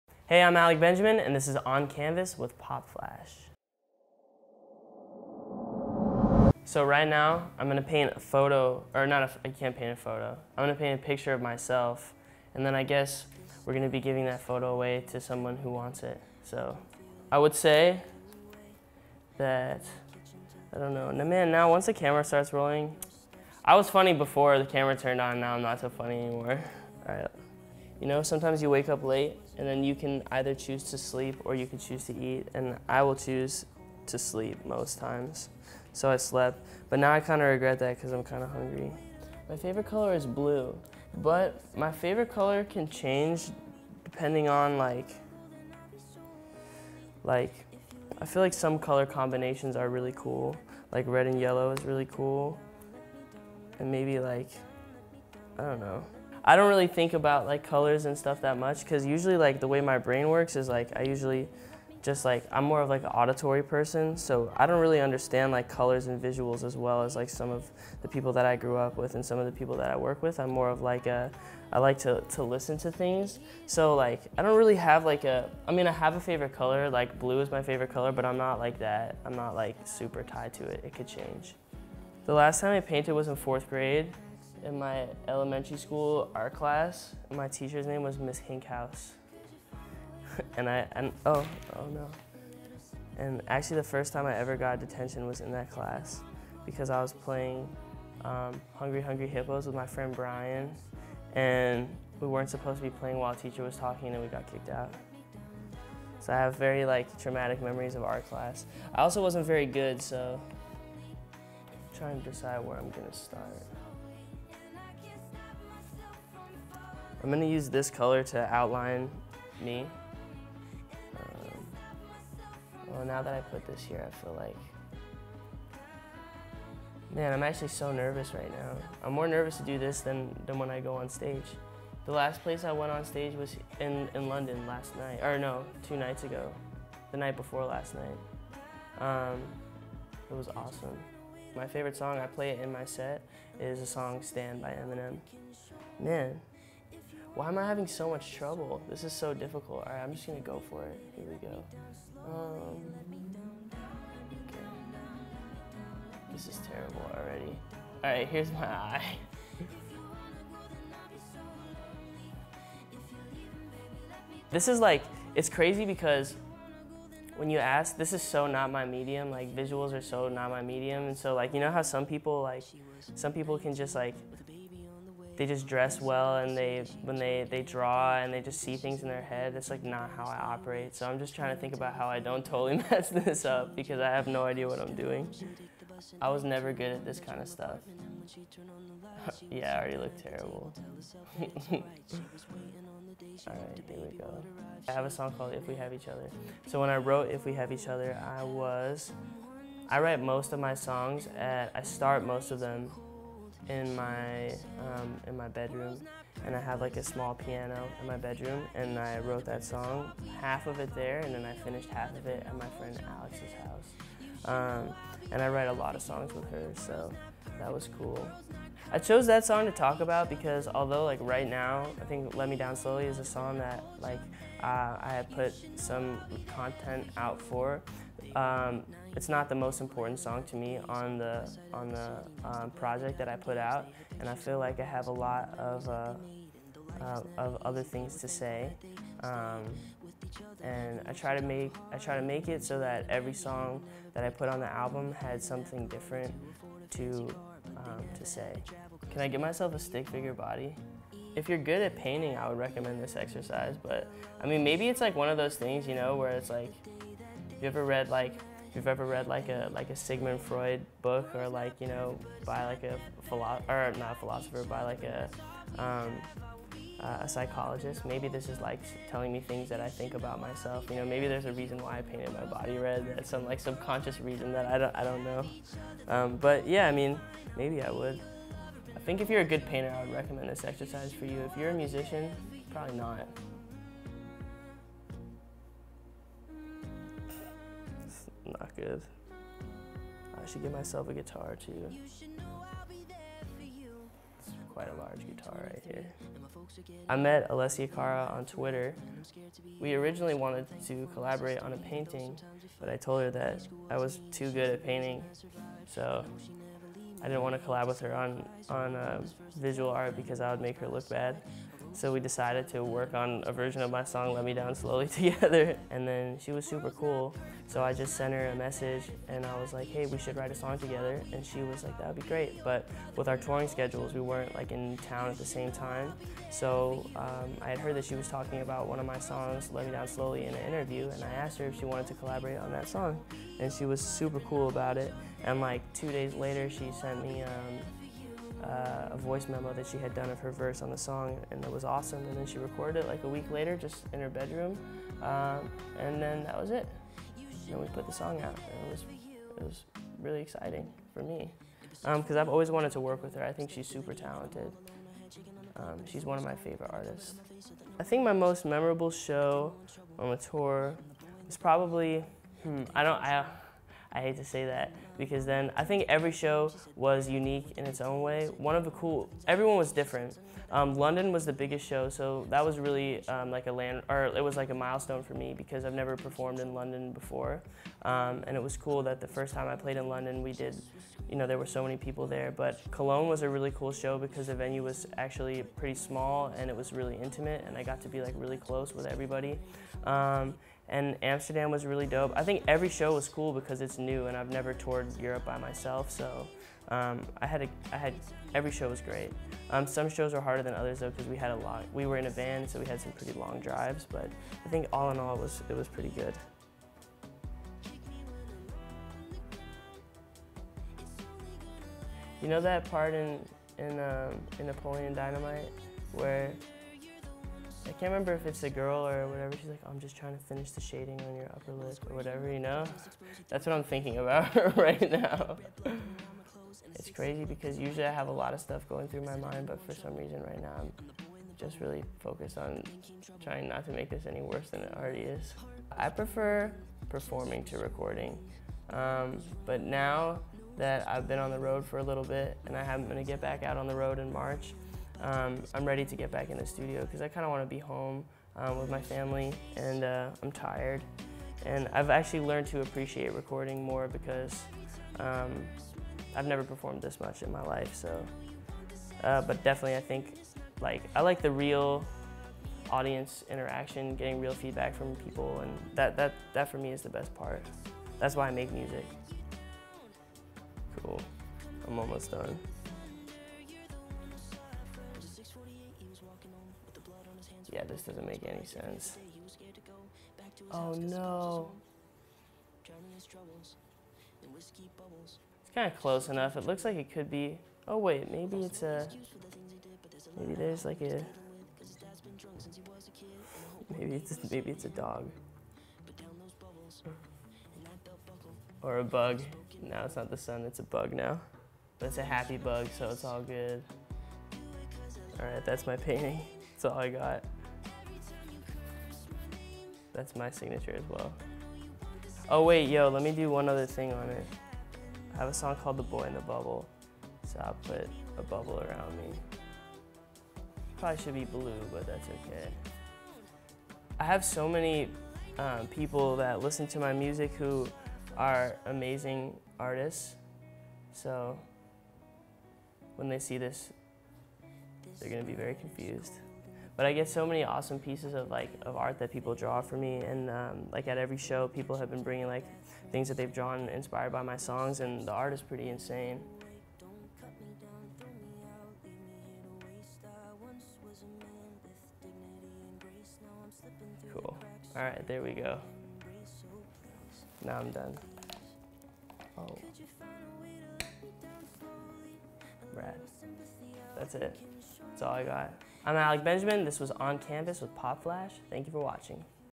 Alec Benjamin | On Canvas
Here is episode 3 in the 'On Canvas' series with rising singer/songwriter, Alec Benjamin.

COMPETITION CLOSED!
Want to win Alec's painted canvas? All you have to do is answer the below question for the chance to win...

Which song does Alec name his painting after?

Competition closes 15th April 2019.